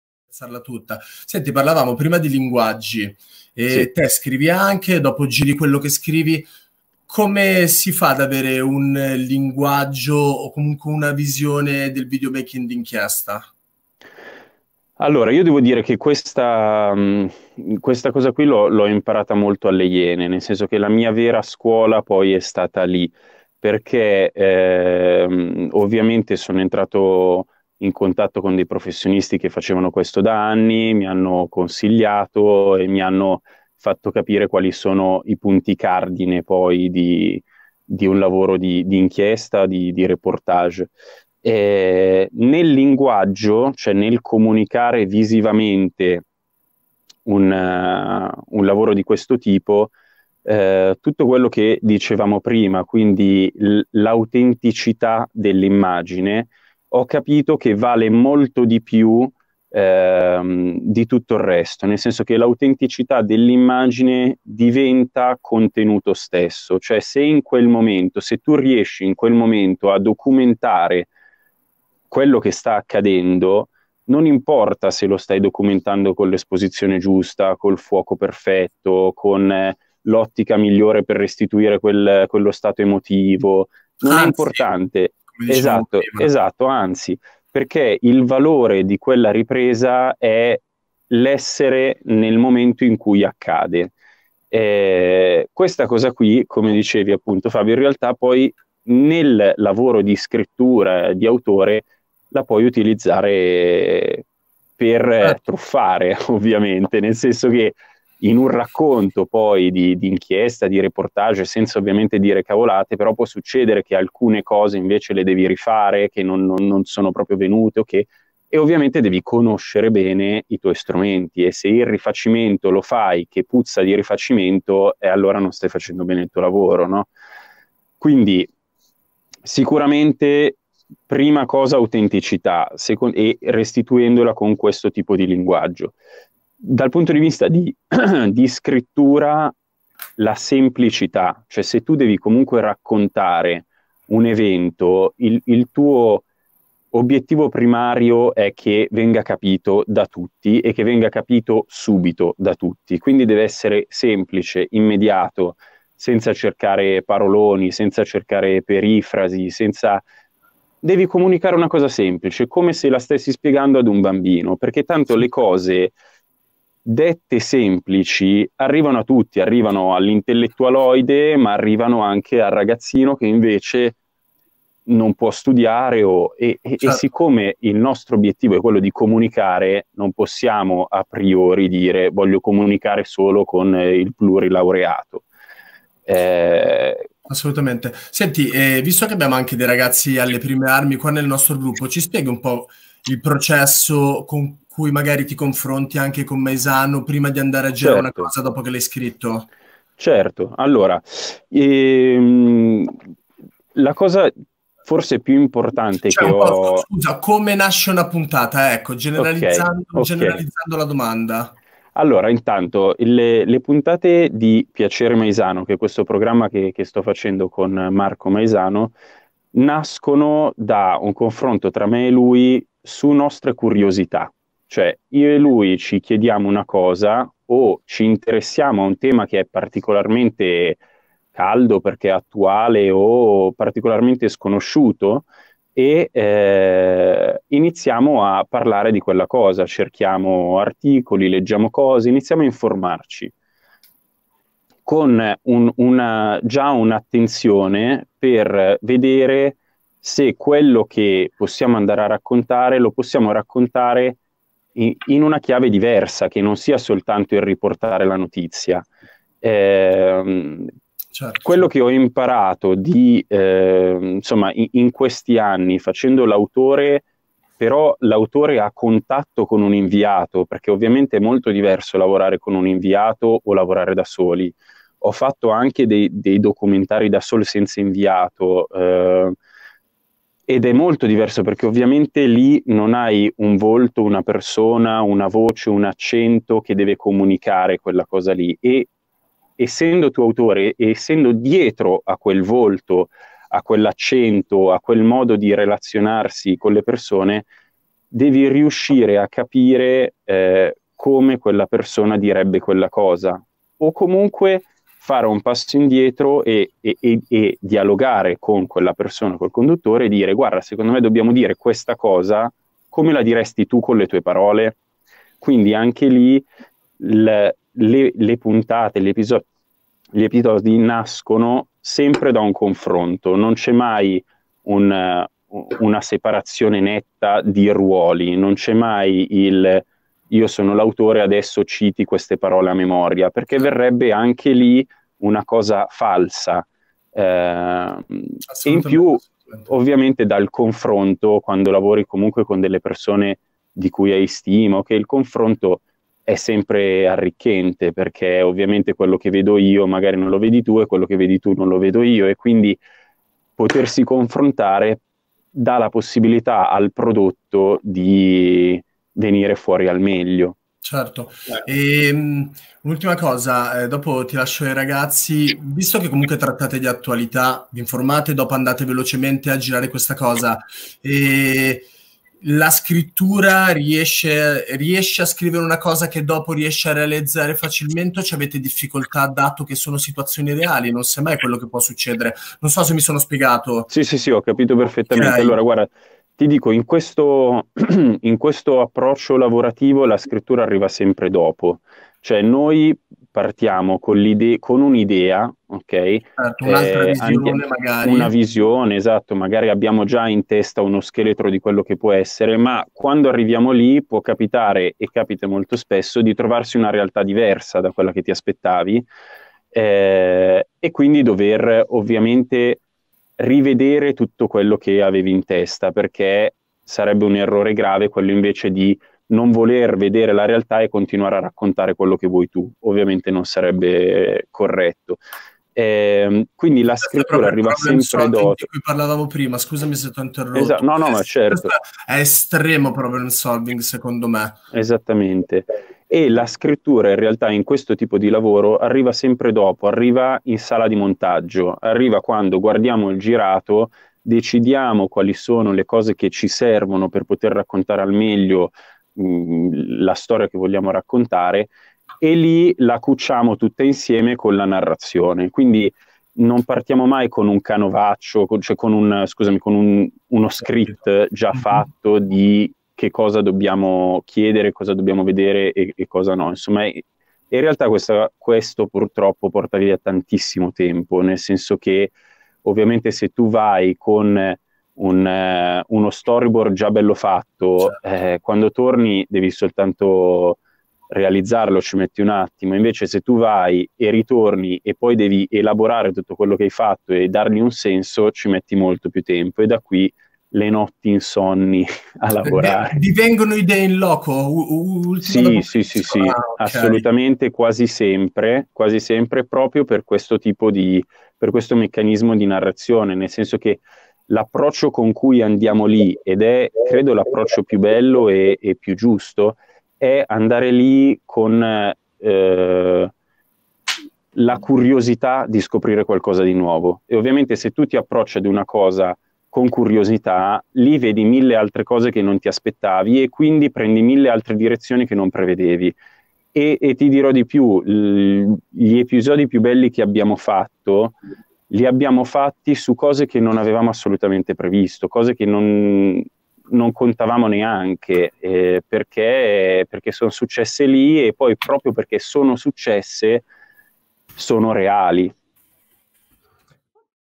IVRS LIVE x Alessandro Casati. Videoreporting creativo
Alessandro Casati è un videoreporter, si è formato alle Iene per dopo approdare come autore/cameraman a Piacere Maisano, programma creato da lui assieme al conduttore Marco Maisano